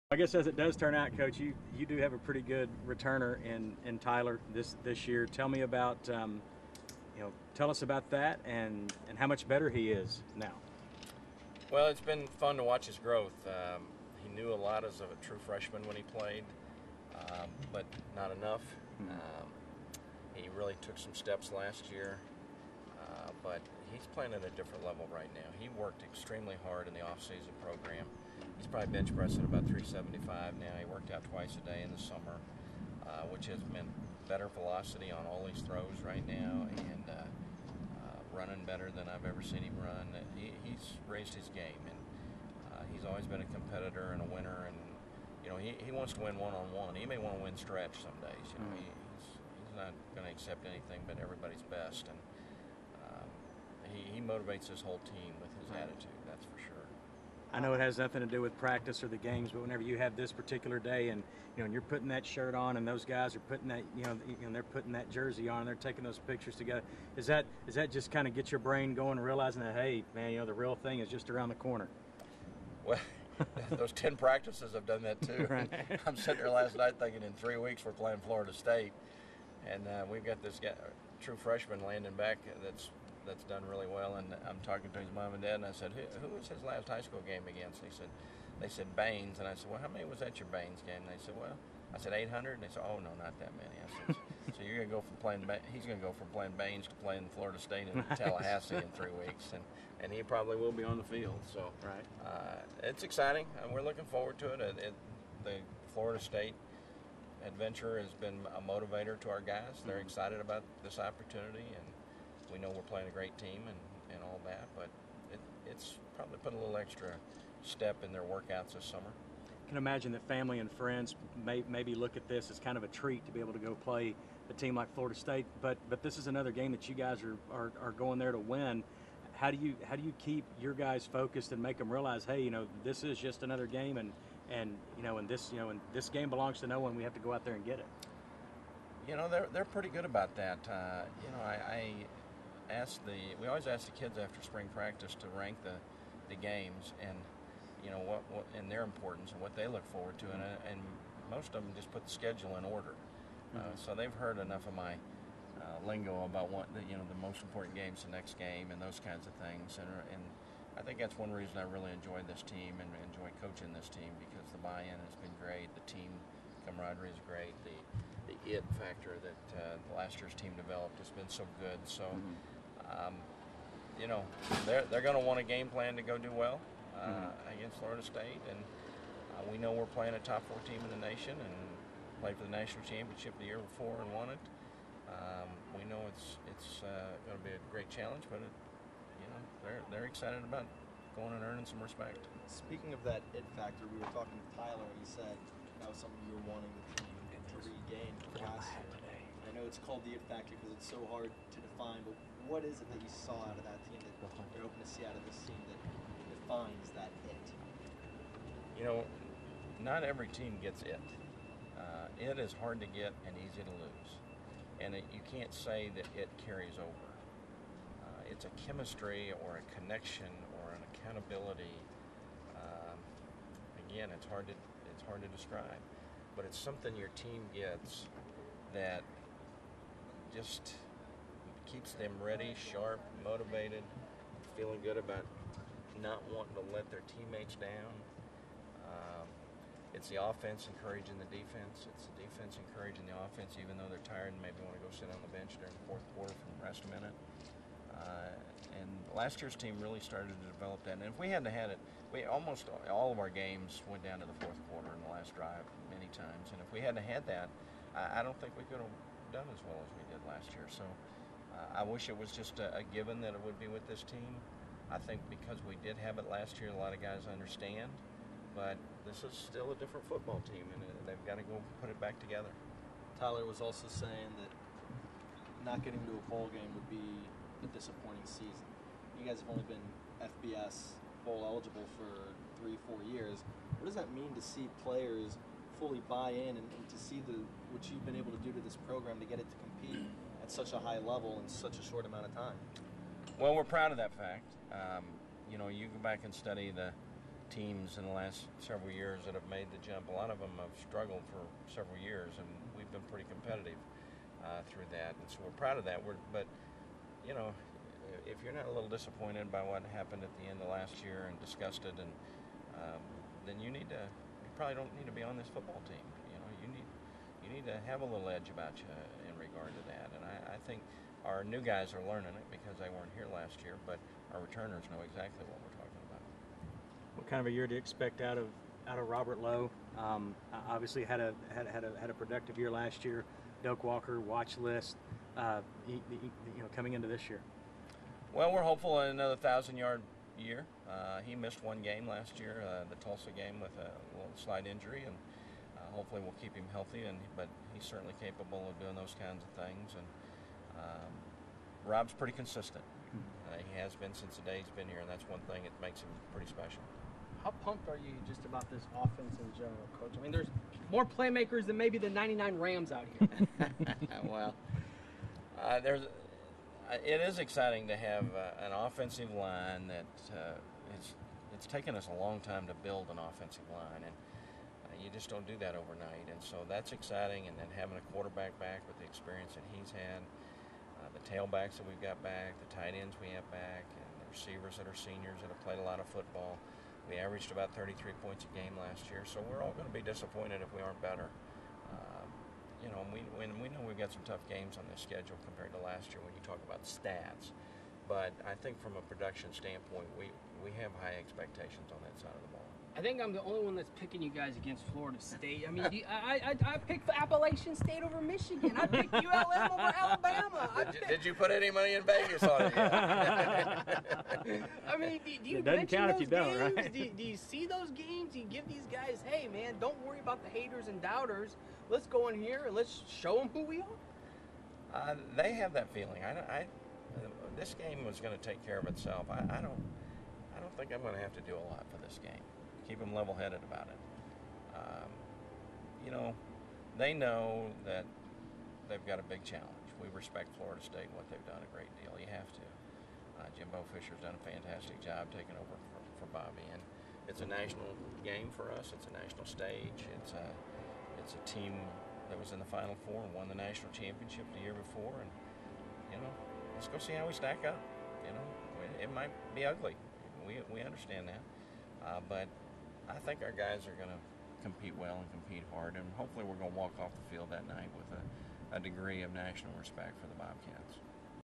Full interview with Dennis Franchione
FOX 7 talks to Texas State's head football coach at Media Days.